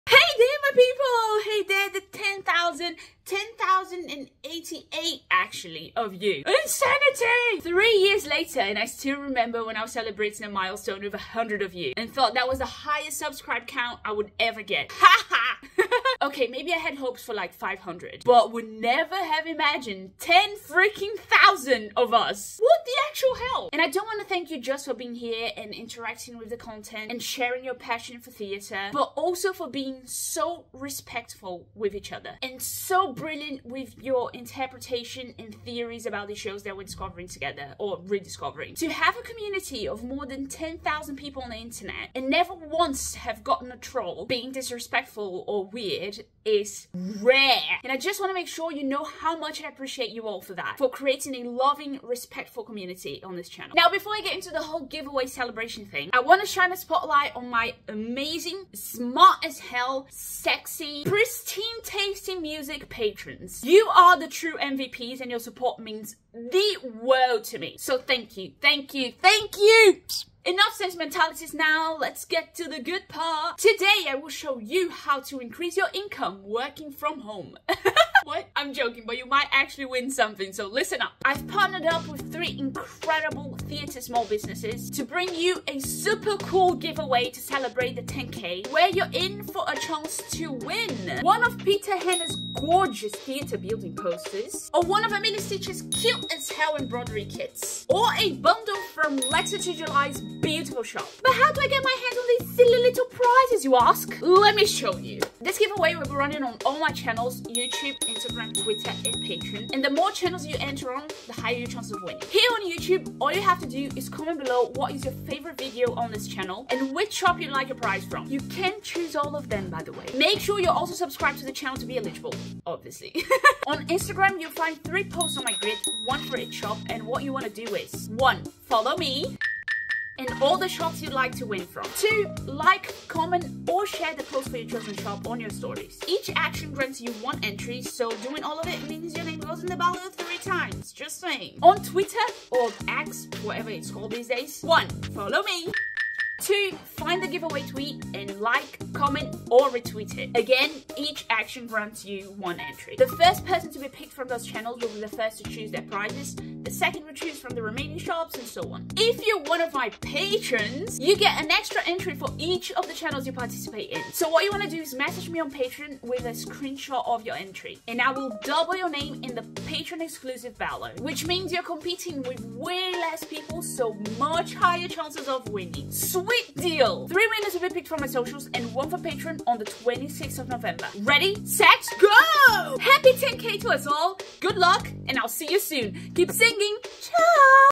10K GIVEAWAY | Celebrating 10K subscribers with theatre prizes galore
Enter the competition for a chance to win prizes from Peter Hannah, Letters to July and Amelia Stitches ❤️ Competition is open for all countries and winners will be announced on the 26th of November.